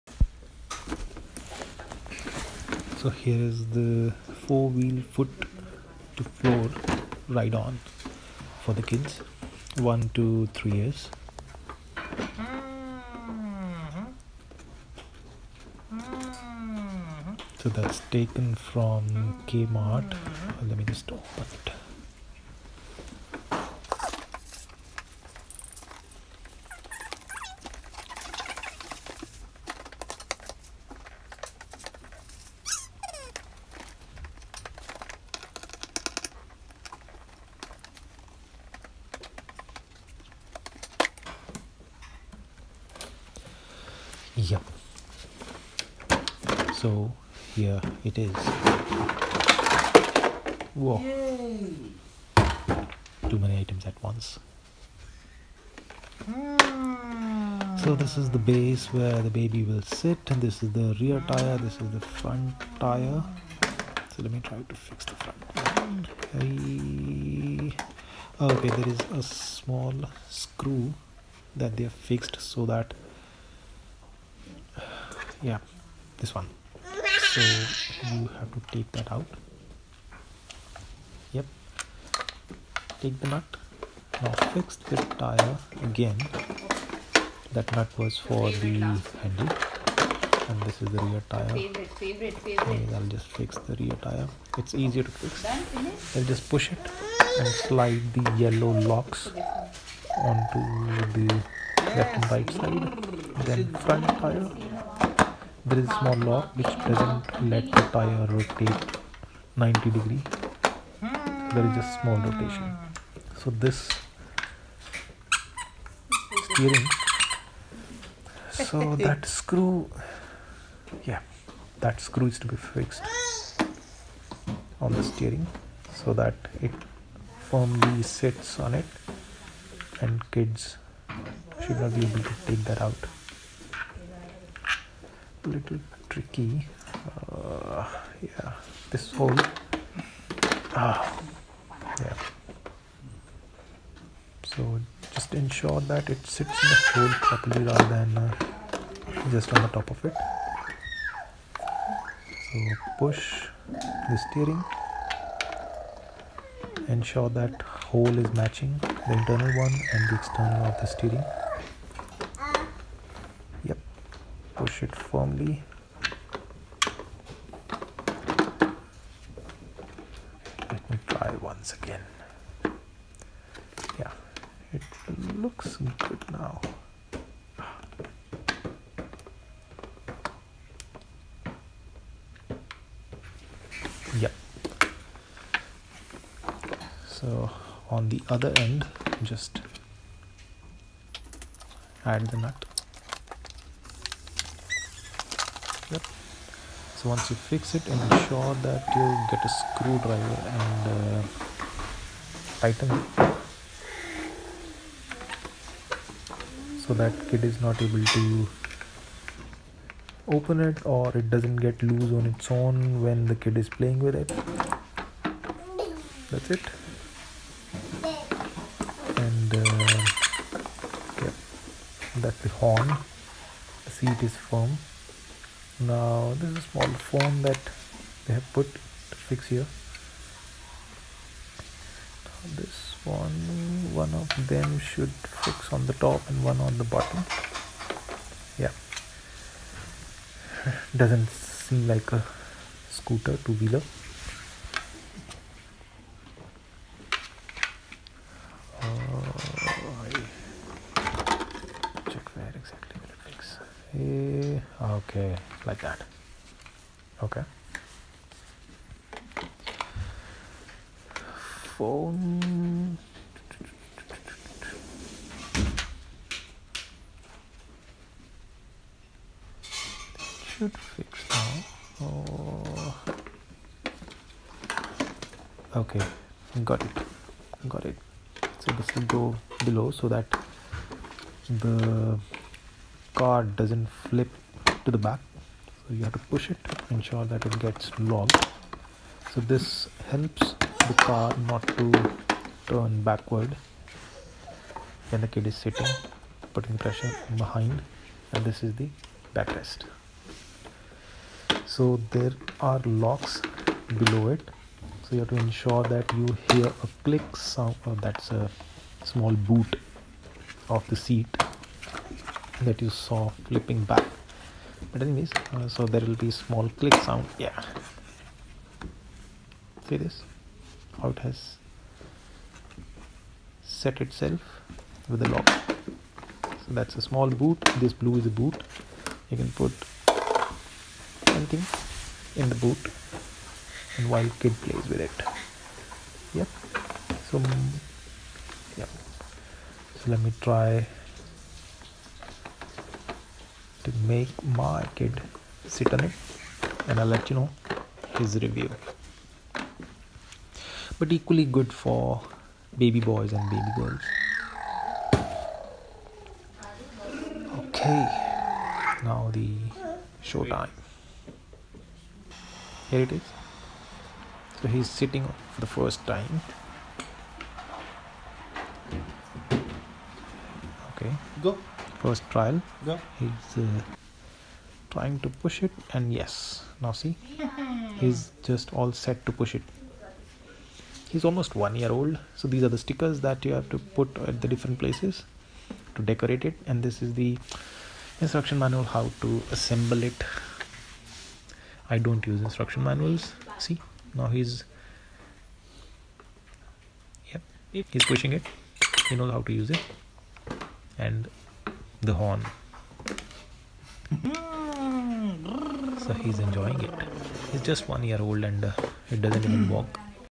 4 Wheel Foot To Floor Ride On | Assemble | Review | Kmart | Kids Toys | 1-3 Years | Anko
Here is assembly and review of four wheel foot to floor ride on toy of kids (1 - 3 years). How to assemble - super easy. Review - your kid will love it. It’s strong yet light weight, easy to drive and stable as well. Do give it to your kid to try.